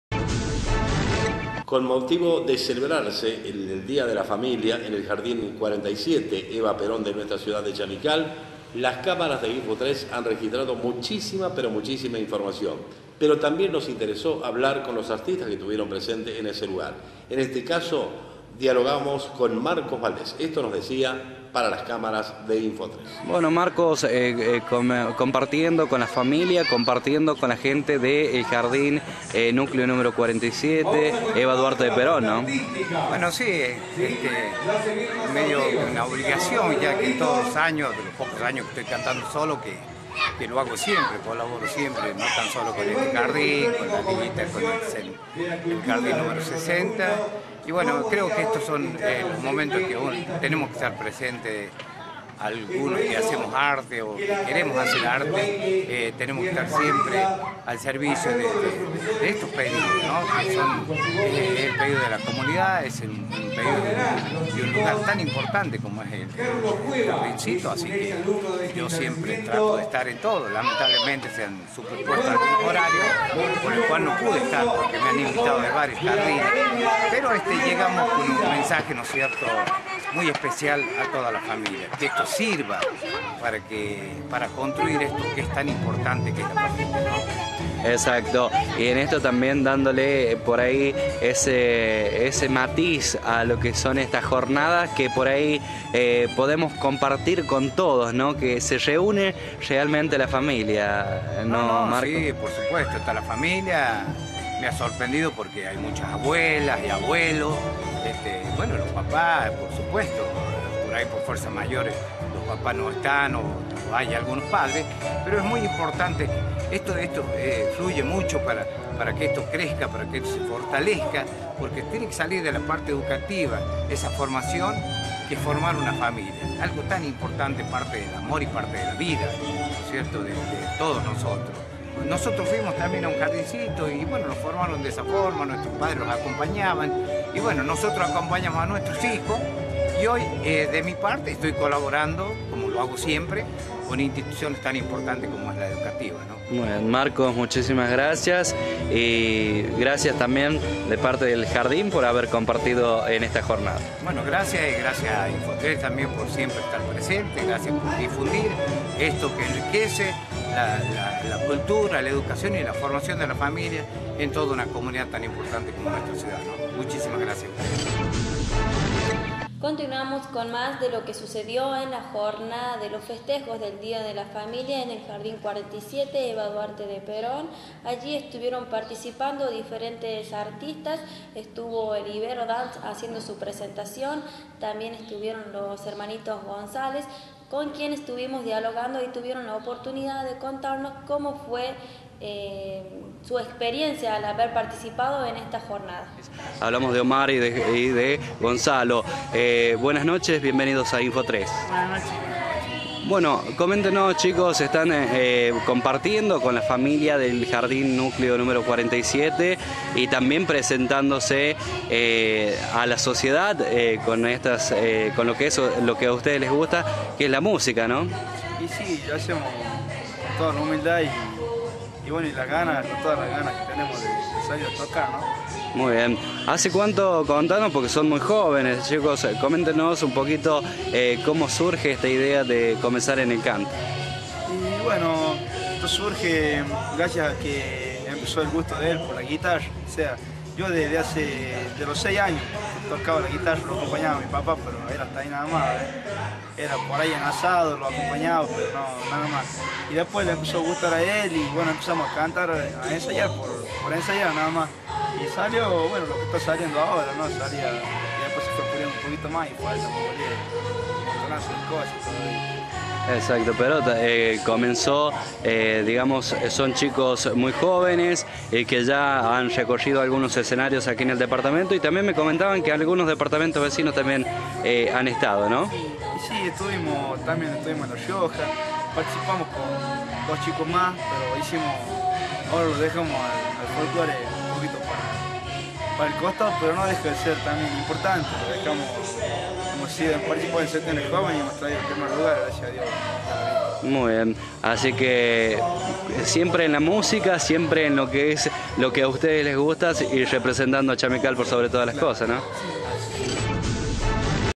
Festejos del día de la familia en el jardín 47
Hablamos con algunos de los artistas que estuvieron compartiendo con el jardín Nº 47 de la ciudad de Chamical.